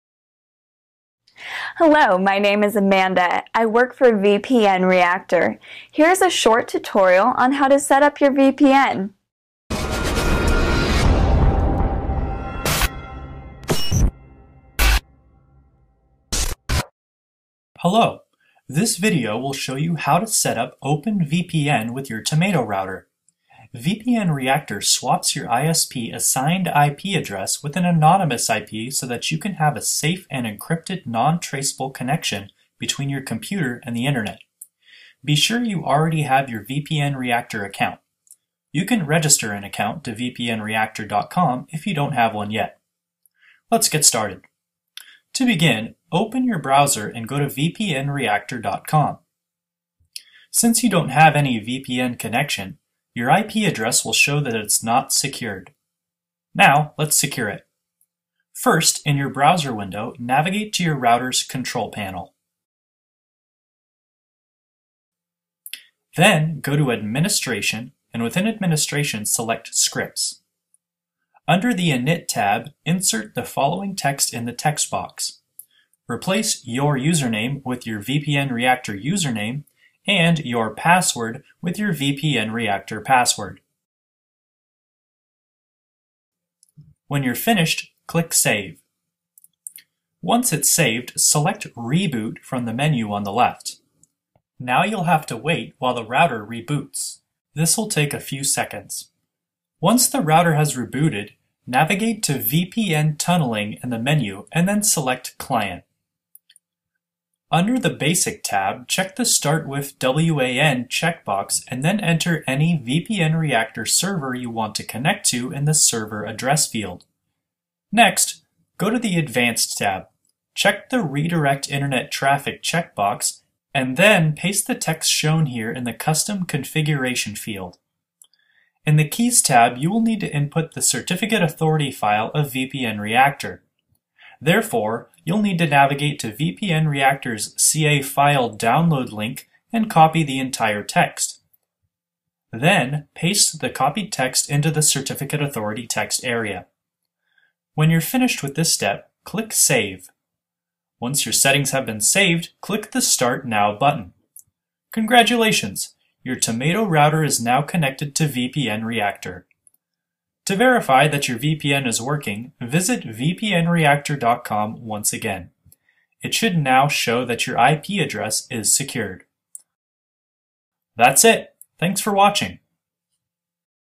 How to setup OpenVPN on a Tomato Router | VPNReactor
- How to setup OpenVPN on a Tomato router.